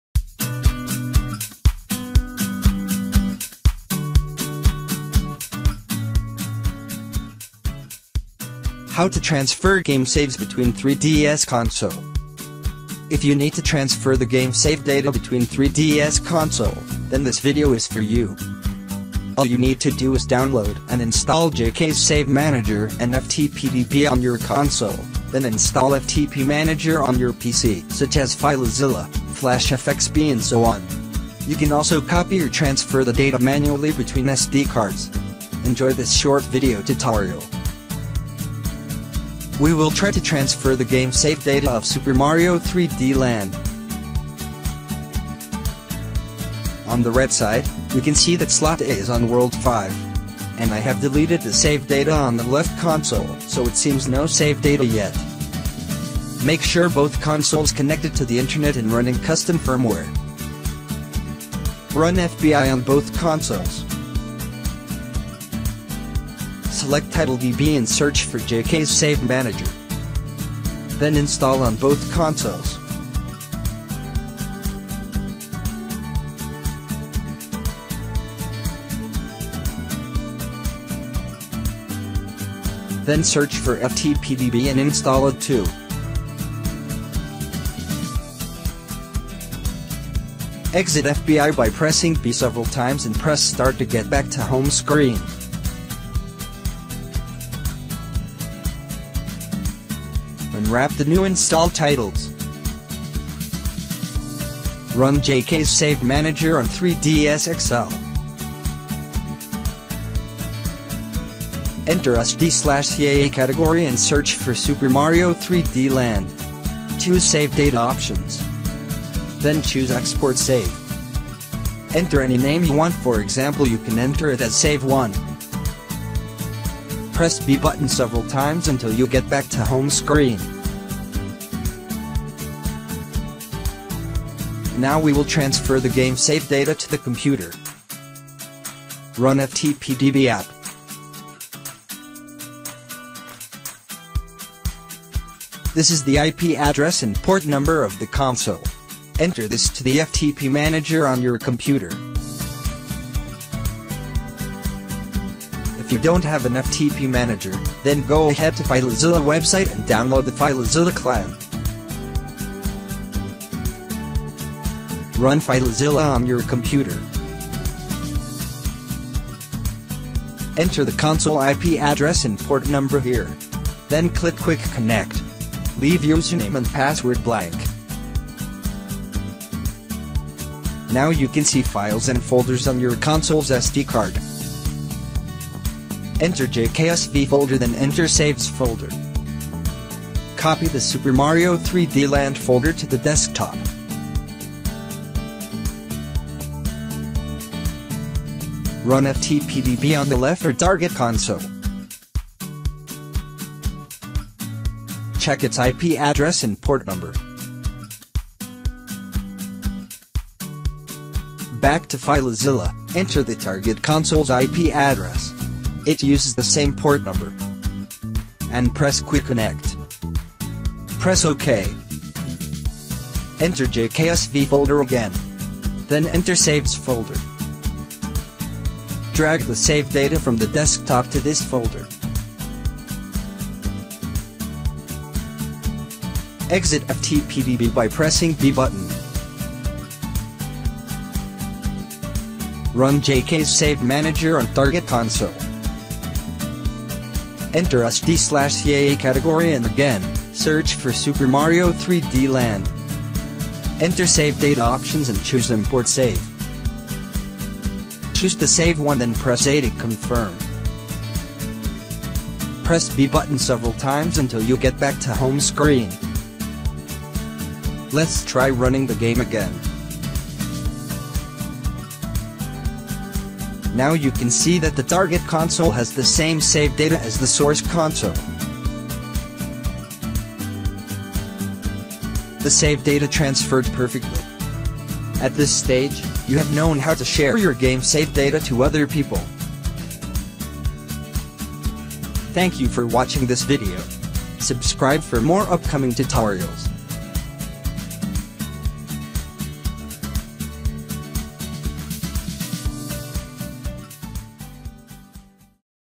How to transfer game saves between 3DS consoles
If you need to transfer the game save data between 3DS console, then this video is for you.

All you need to do is download and install JK's save manager and FTPDB on your console, then install FTP Manager on your PC (such as Filezilla, FlashFXP and so on). You can also copy or transfer the data manually between sd cards.

Enjoy this short video tutorial.

LINKS

MUSIC